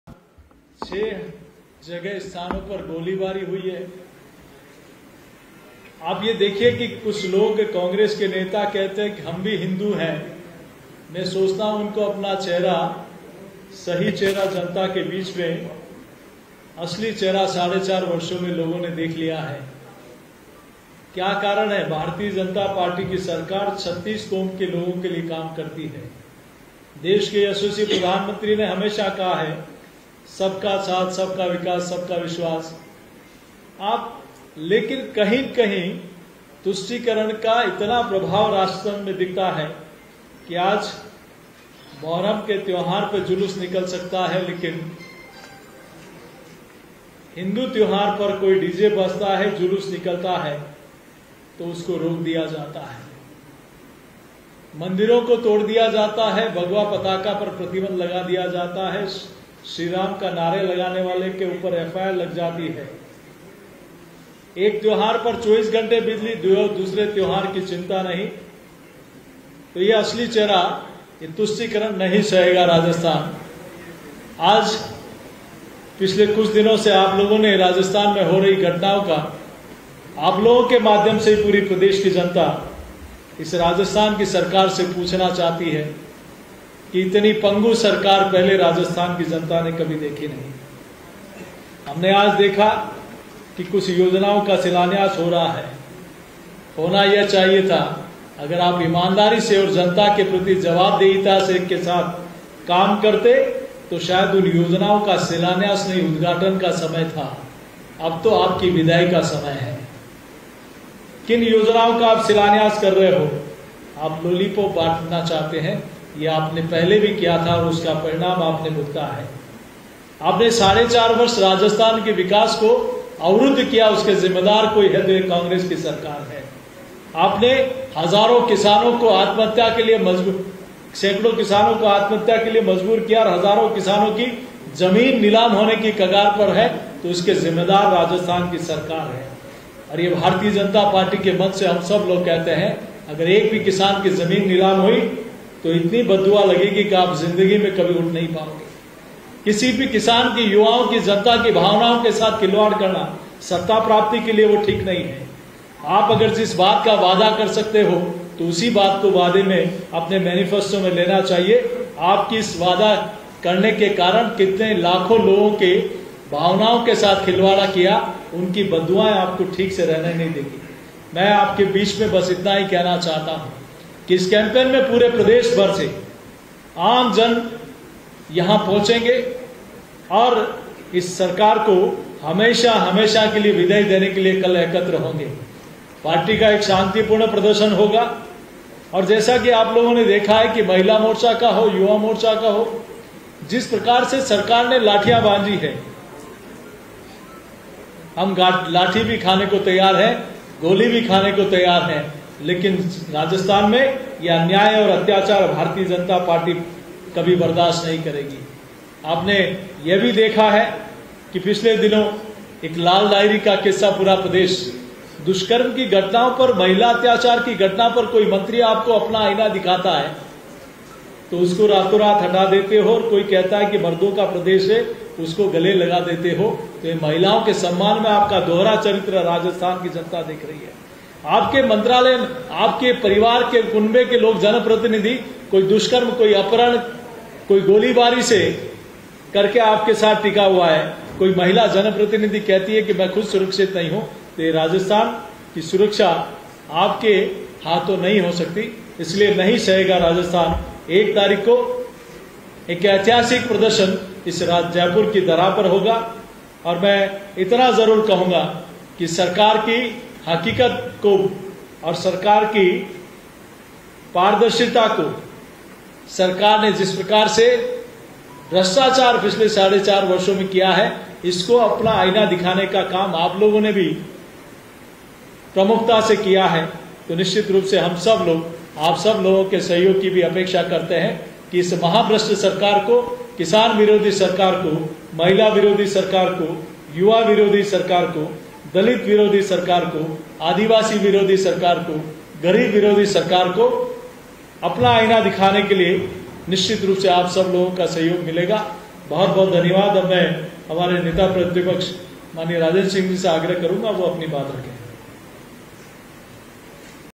CP Joshi Live | बीजेपी मुख्यालय में प्रेस वार्ता, 1 अगस्त को बीजेपी का सचिवालय महाघेराव | JAN TV
Watch JAN TV on :
Tata Play DTH : 1185
Airtel DTH: 355
JIO Fiber: 1384

News Credit-MKP